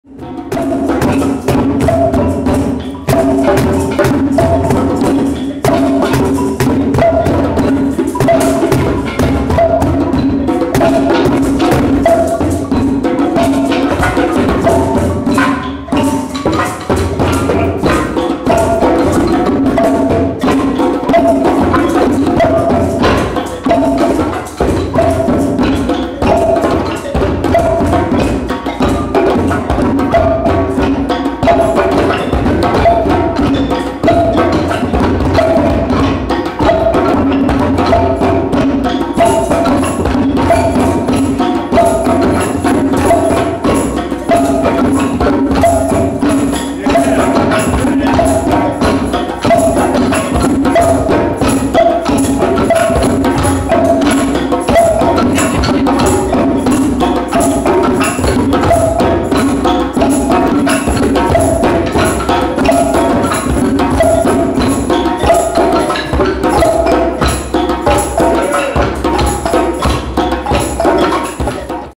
Music is the Healing Force of The Universe MEDEA
Music is the Healing Force of the Universe Orchestra has kicked in full force. Idris Ackamoor directing THE MEDEA PROJECT: THEATER FOR INCARCERATED WOMEN component of the Orchestra. The Medea Project is under the directorship of Rhodessa Jones and is composed of ex- inmates, HIV positive Women from the Women's HIV Program at UCSF, and community actresses. This rehearsal is focused on the women playing the djembe drum in combination with African balaphons and other percussions. Fe Bongolan, Lisa Frias, and Tara Paulson are playing th djembe drums, Felicia Scaggs, Polina Smith, and Mary Shellhammer, Marlene Stoeckl, Cassidy, and Cassandra Steptoe are playing percussions.  This first rehearsal was to establish a rhythm vocabulary by which the group can proceed as well as to familiarize the participants with each instrument. The project is supported by the James Irvine Foundation Exploring Engagement Program.